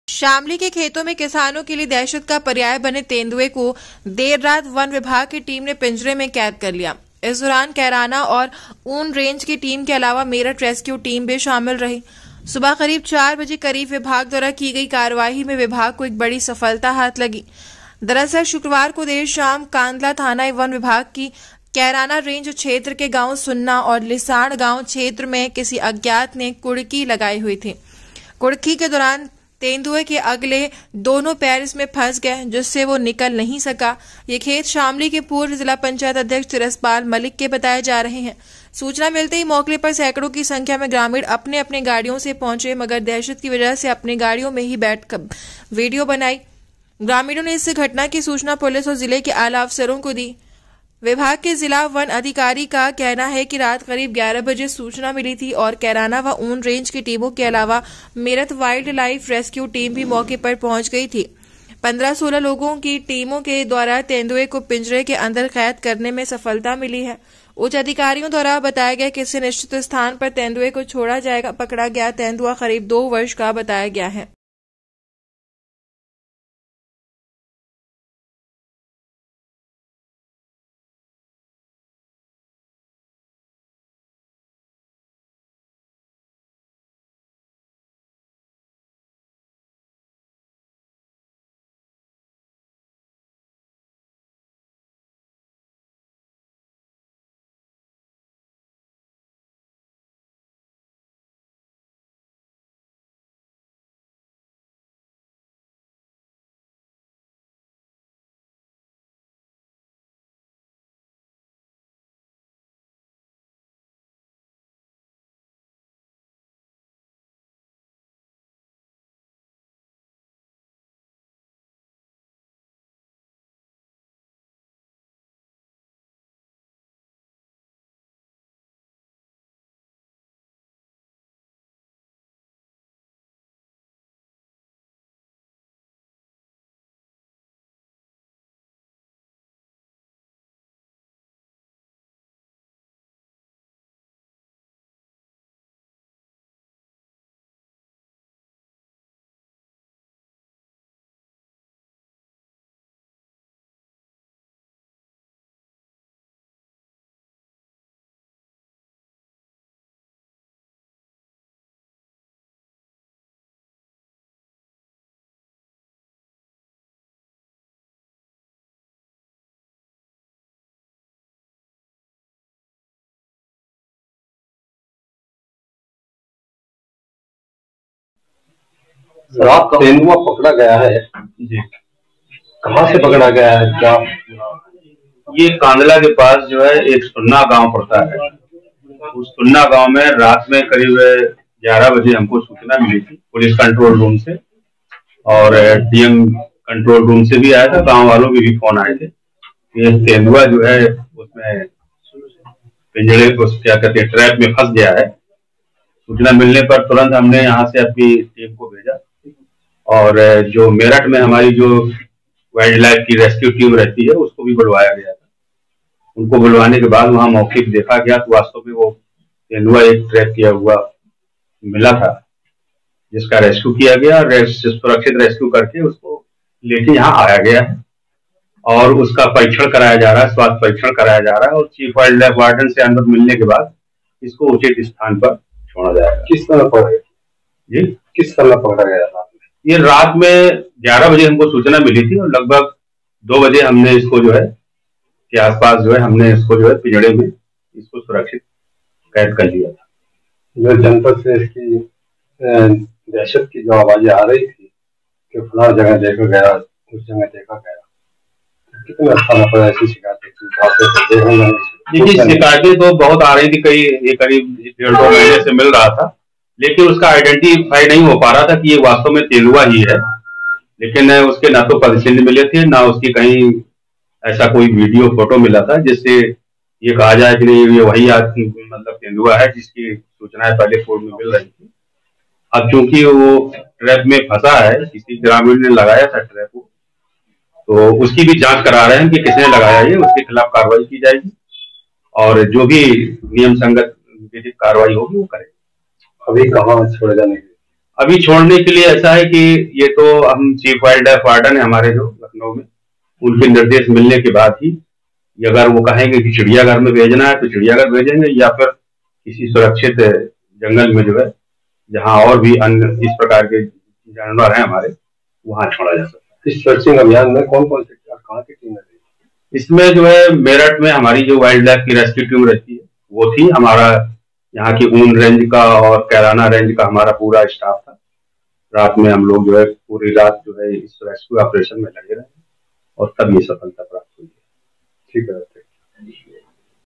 जिले में किसानों की दहशत का पर्याय बना तेंदुआ।
जिले में किसानों की दहशत का पर्याय बना तेंदुआ, देर रात वन विभाग की कार्यवाही में पकड़ा गया
*  जिले में भड़ी, सूबरी, और भैंसवाल सुन्ना में तेंदुए को देखे जाने की मिल रही थी सूचनाएँ
* जिले में तेंदुए से किसानों की दहशत खत्म होने का वन विभाग का दावा
शामली के खेतों में किसानों के लिए दहशत का पर्याय बने तेंदुए को देर रात वन विभाग की टीम ने पिंजरे में कैद कर लिया। इस दौरान कैराना और ऊन रेंज की टीम के अलावाY मेरठ रेस्क्यू टीम भी शामिल रही। सुबह करीब 4 बजे करीब विभाग द्वारा की गई कार्यवाही में विभाग को एक बड़ी सफलता हाथ लगी। दरअसल शुक्रवार देर शाम कांधला थाना एवं वन विभाग की कैराना रेंज क्षेत्र के गांव सुन्ना और लिसाढ गांव खेतों में किसी अज्ञात ने कुड़की लगाई हुई थी। कुड़की के दौरान तेंदुए के अगले दोंनो पैर इसमें फंस गए । जिससे वह निकल नहीं सका। यह खेत शामली के पूर्व जिला पंचायत अध्यक्ष तिरसपाल मलिक के बताए जा रहे हैं। सूचना मिलते ही मौके पर सैकड़ो की संख्या में ग्रामीण अपने-अपने गाड़ियों से पहुंचे मगर दहशत की वजह से अपनी गाड़ियों में ही बैठकर वीडियो बनाई।
     ग्रामीण ने इस घटना की सूचना पुलिस और जिले के आला अफसरों को दी। विभाग के जिला वन अधिकारी का कहना है कि रात करीब 11 बजे सूचना मिली थी। और कैराना व ऊन रेंज की टीमों के अलावा मेरठ वाइल्डलाइफ रेस्क्यू टीम भी मौके पर पहुंच गई थी। 15 - 16 लोगों की टीमों के द्वारा तेंदुए को पिंजरे के अंदर कैद करने सफलता मिली है। उच्च अधिकारियों द्वारा बताए गए किसी निश्चित स्थान पर तेंदुए को छोड़ा जाएगा। पकड़ा गया तेंदुआ करीब 2 वर्ष का बताया गया है।